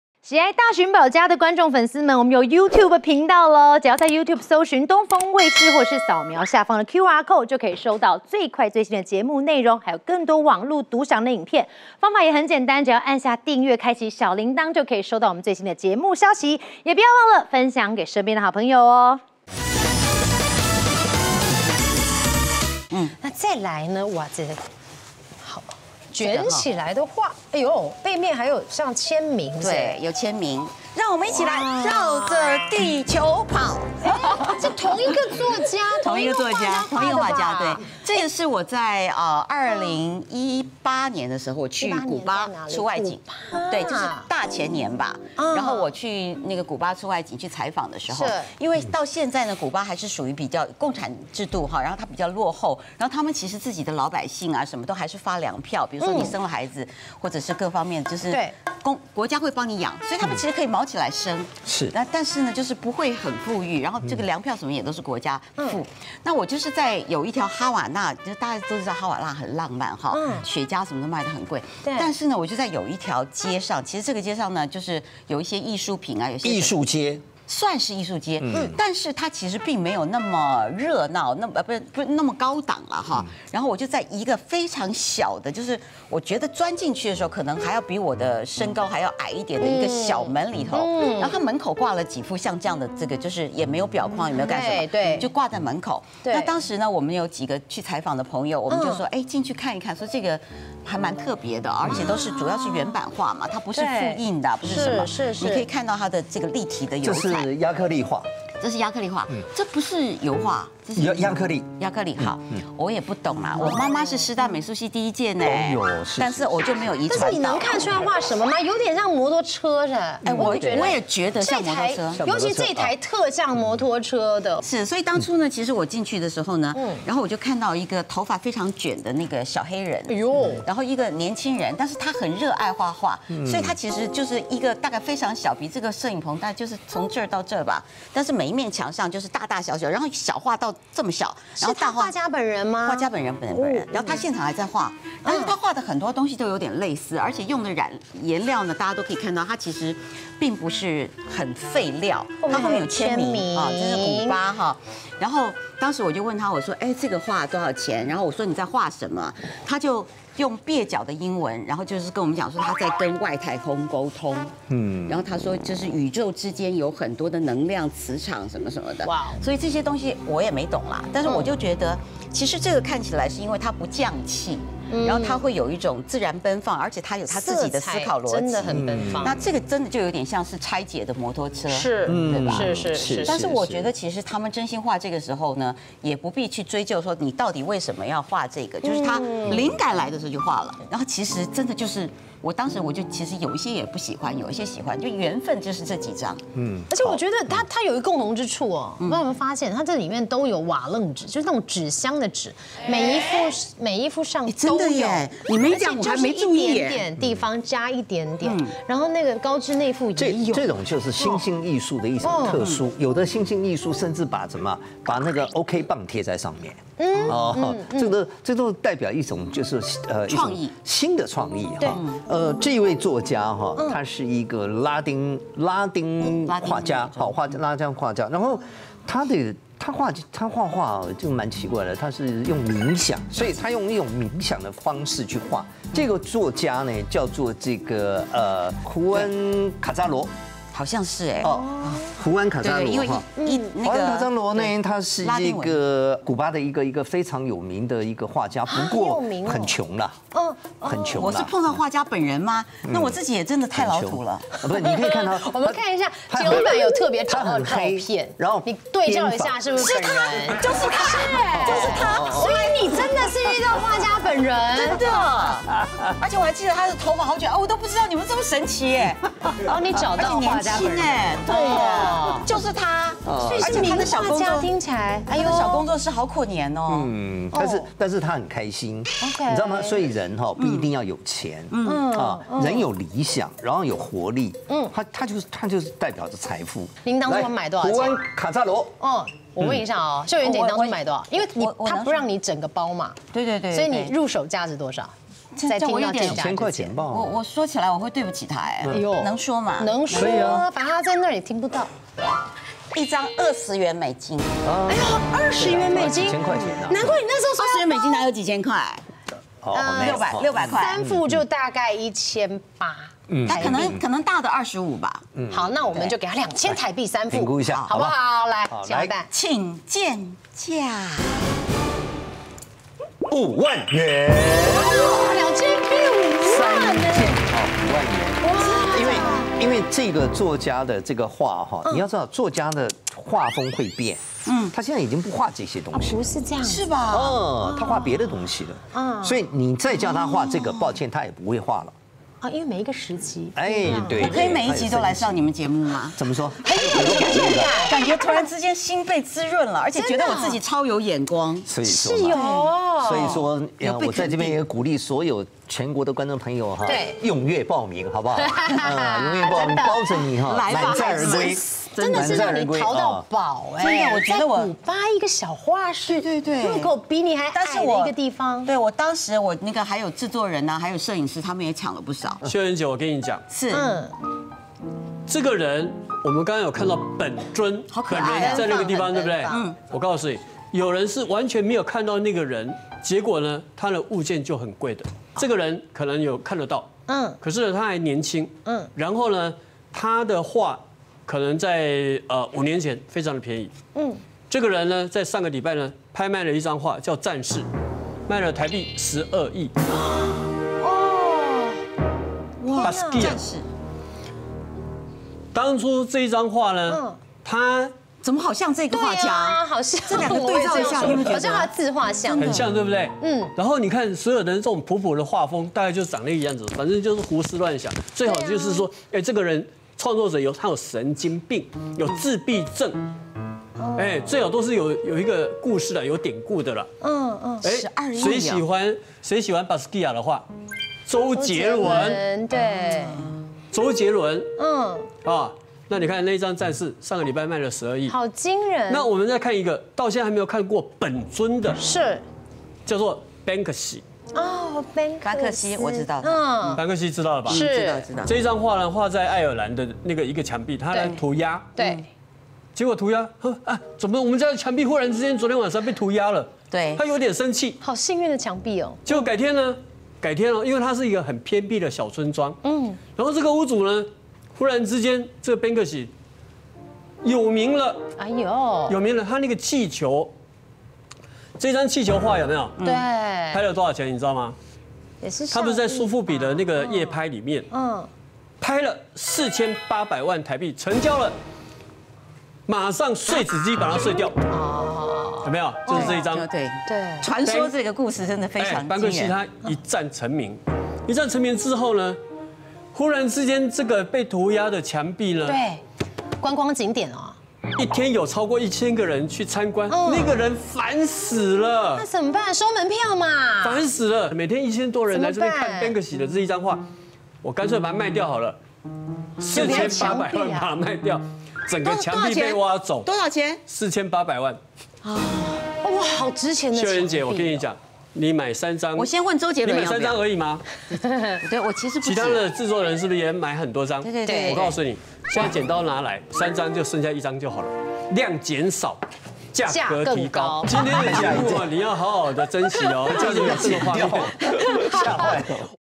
【精華版】12億天價驚全場!超強收藏潛力股
外景旅遊時買下的畫作，沒想到價格成長了。